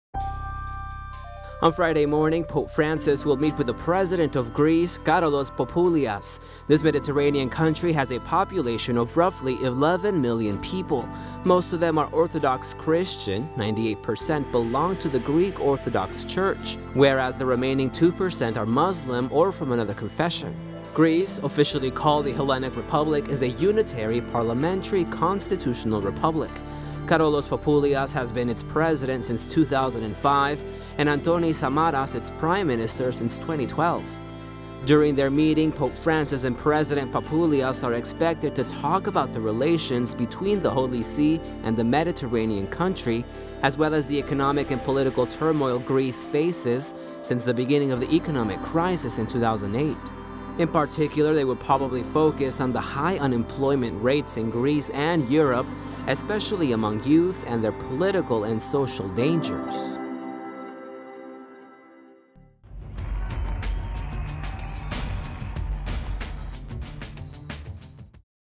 Pope will meet Karolos Papoulias, President of Greece
March 27, 2014. (Romereports.com) This Mediterranean country has a population of roughly 11 million people. Most of them are Orthodox Christians: 98 percent belong to the Greek Orthodox Church, whereas the 2 percent left are Muslims or from another confession. .